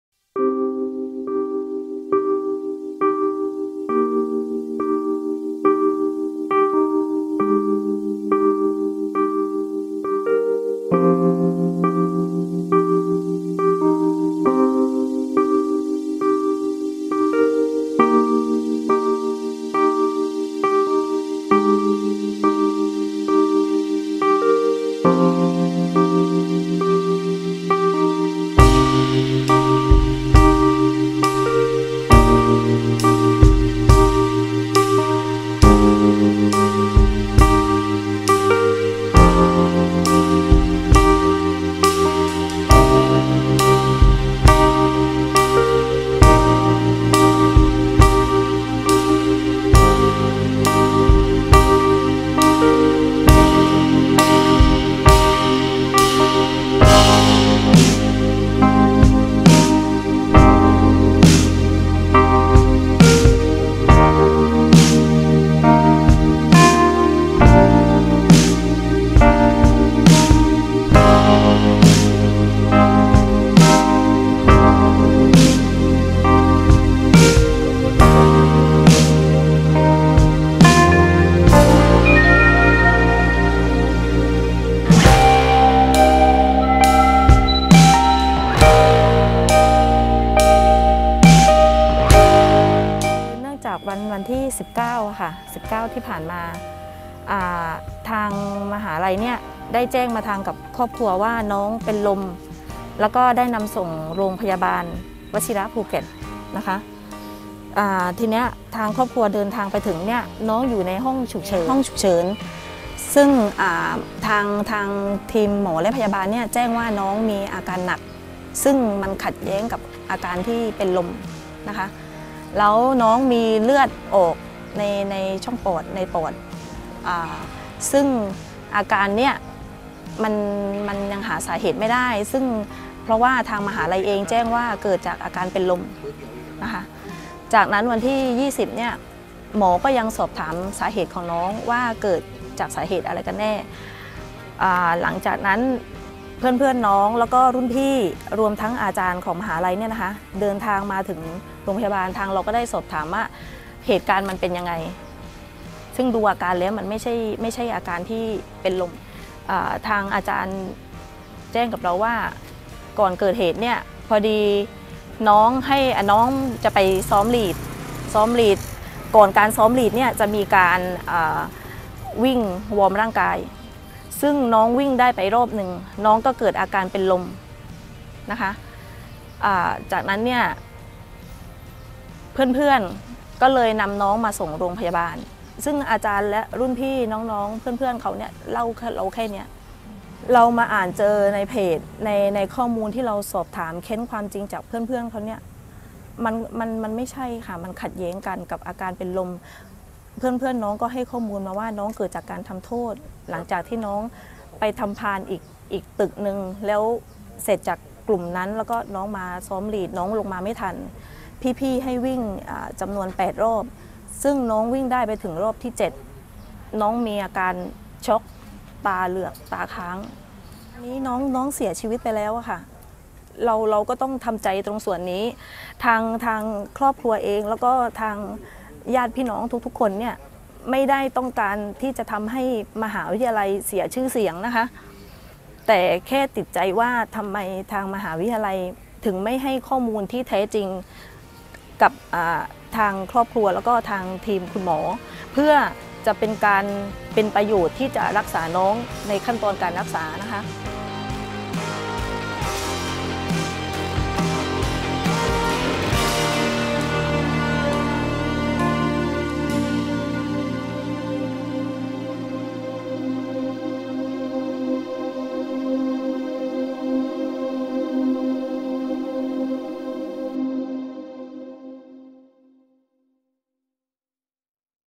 เศร้า! "น้องมิ้น มรภ.ภูเก็ต" ถูกรุ่นพี่ลงโทษวิ่งรอบชุมน้ำช็อกดับ : เรื่องเด่นทั่วไทย
ญาติทำใจไม่ได้หลังทราบ “น้องมิ้น พรพิพัฒน์ เอียดดำ” นักศึกษาชั้นปีที่ 1 คณะมนุษยศาสตร์และสังคมศาสตร์ มหาวิทยาลัยราชภัฏภูเก็ต เสียชีวิตเพราะถูกรุ่นพี่ทำโทษแค่ไปซ้อมเชียร์สาย สั่งวิ่ง 8 รอบขุมน้ำขนาดใหญ่จนช็อกแต่แจ้งว่าเป็นลม ชี้ทำไมไม่บอกความจริงหมอเพื่อประโยชน์ในการรักษา รุดแจ้งความแล้ว